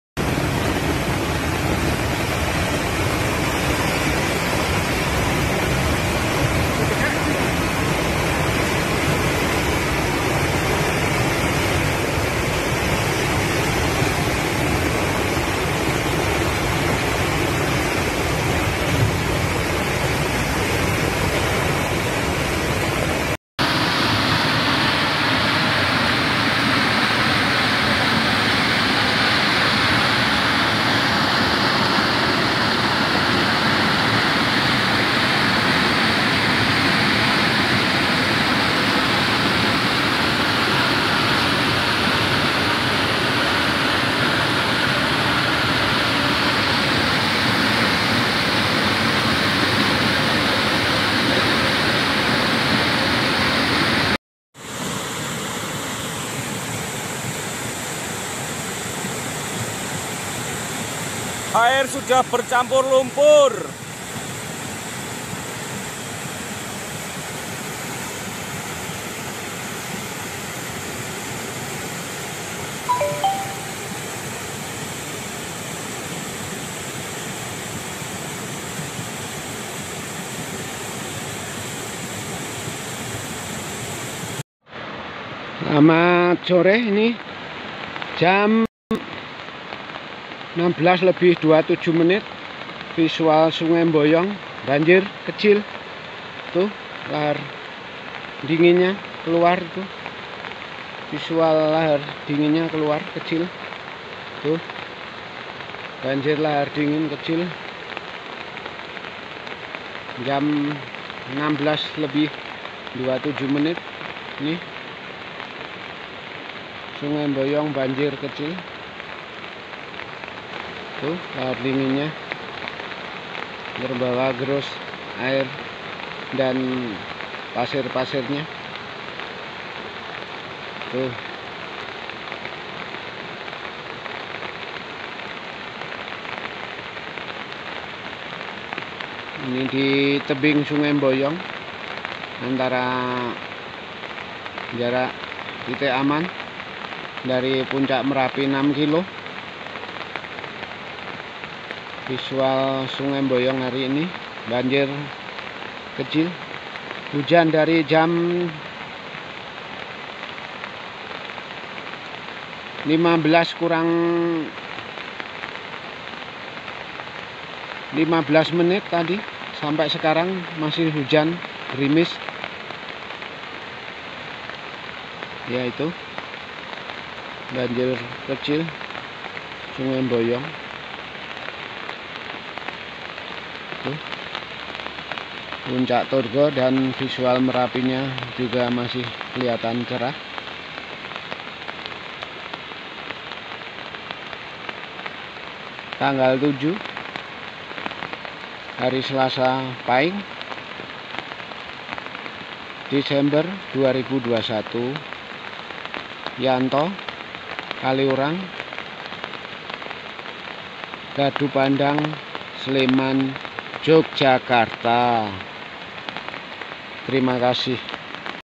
Pantauan Lahar Dingin Merapi • 07 Desember 2021
Pantauan visual dari rekan Relawan

Titik pantau Kali Gendo,Kali Kuning dan Kali Boyong